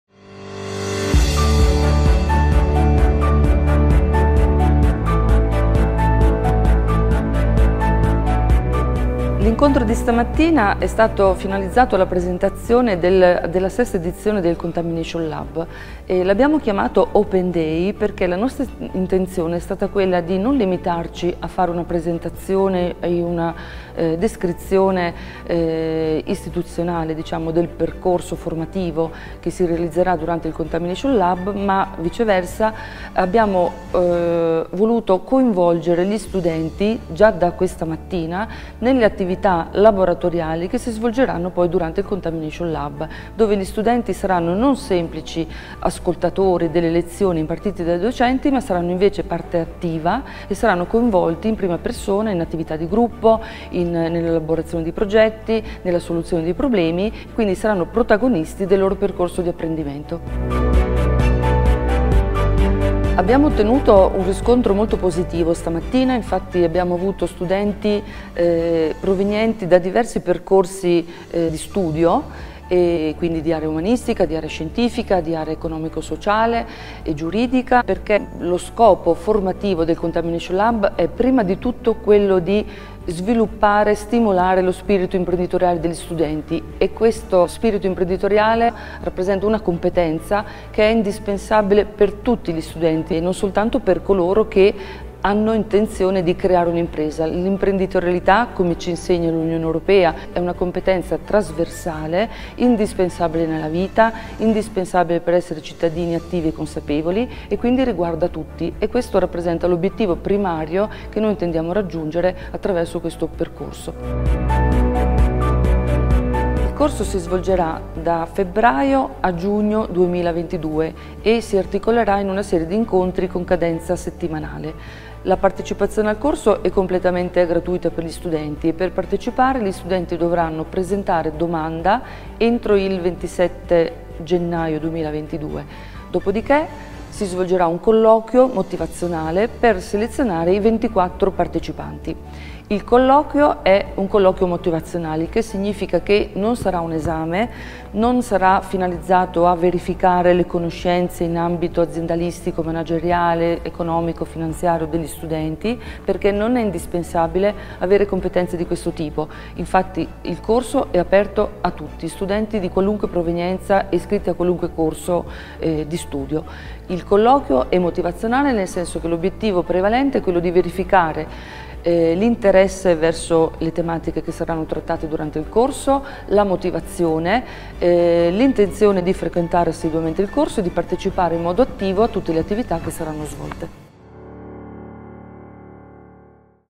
Sesta edizione del Contamination Lab Uniurb
Perché e come partecipare a Uniurb Lab 6? A spiegarlo è la Professoressa Francesca Maria Cesaroni, docente di Economia Aziendale, responsabile del coordinamento scientifico del Contamination Lab dell'Università degli Studi di Urbino.

Visita il sito e scarica il bando!
Le iscrizioni scadono il 27 gennaio 2022.